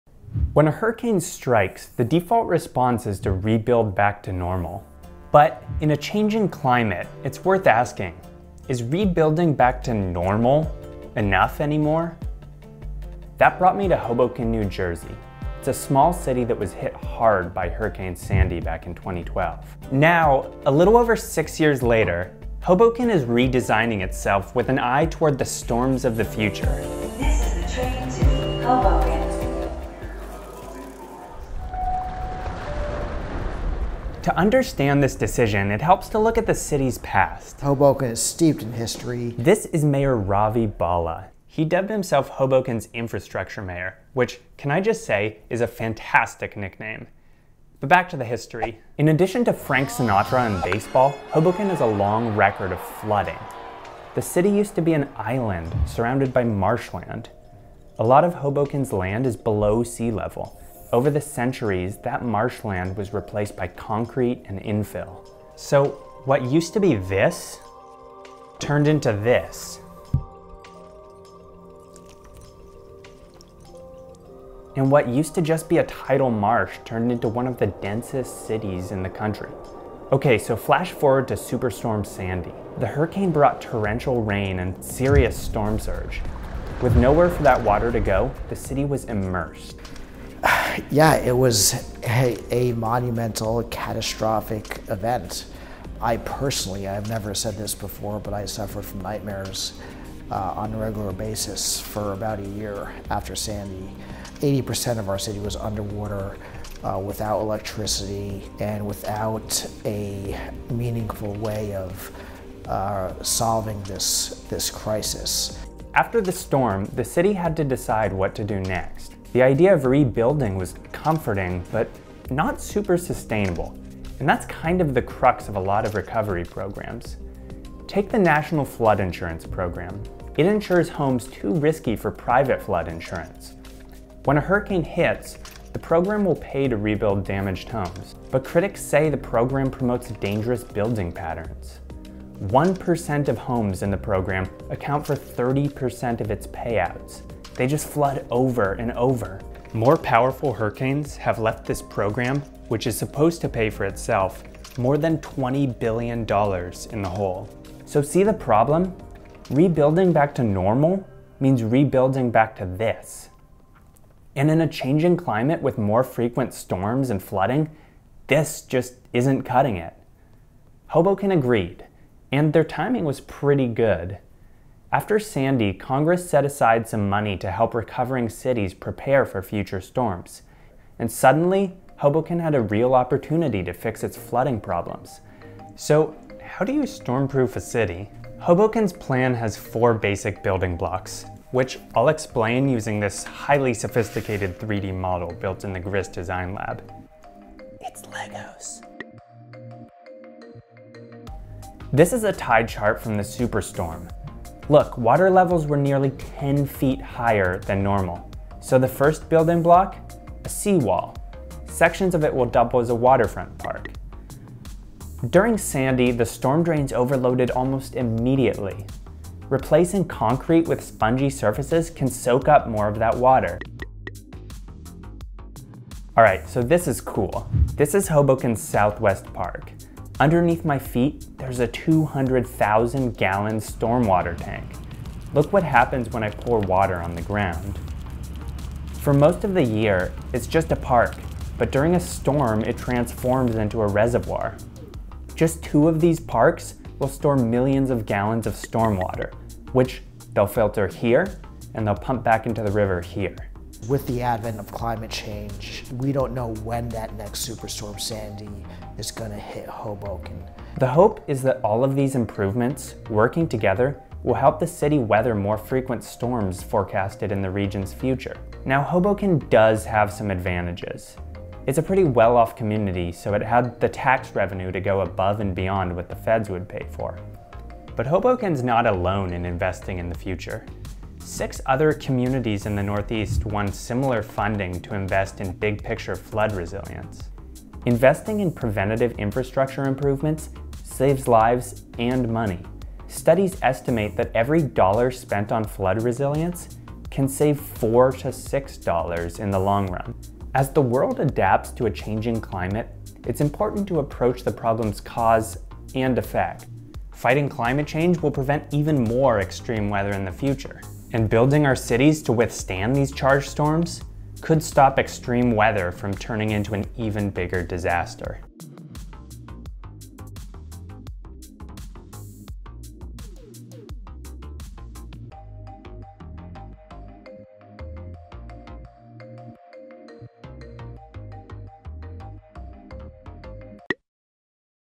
Inside a city redesigned for superstorms and sea level rise
Six years after Hurricane Sandy, Hoboken, New Jersey, is redesigning itself with an eye toward the storms of the future.

Sources:

City of Hoboken
Mayor Ravi Bhalla

Chris Currie, GAO